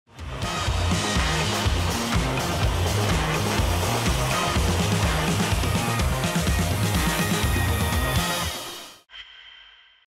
F-35B Shipborne Rolling Vertical Landing (SRVL) HMS Prince of Wales
📽️: The F-35B Lightning II completed its first shipborne rolling vertical landing (SRVL) aboard HMS Prince of Wales off the U.S. Eastern Seaboard.